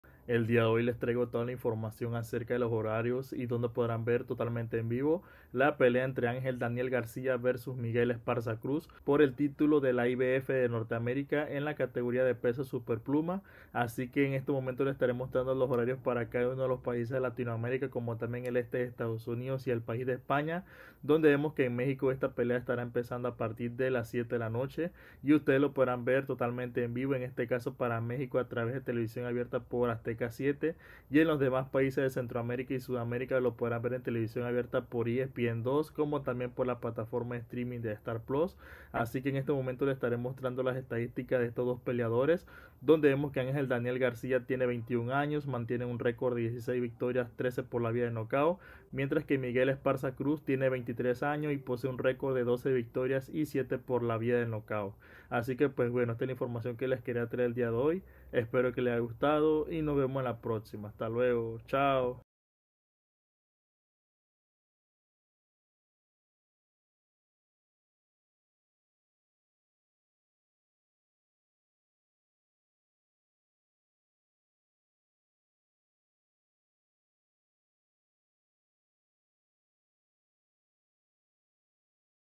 🚨BOXEO🚨Dónde Ver EN VIVO La Pelea de Angel Daniel Garcia vs Miguel Esparza Cruz por el Título IBF
En este video les muestro la información, horarios, estadísticas y donde podrán ver en vivo de la pelea entre Angel Daniel Garcia vs Miguel Esparza Cruz por el Título IBF de Norteamérica en la categoría de peso superpluma.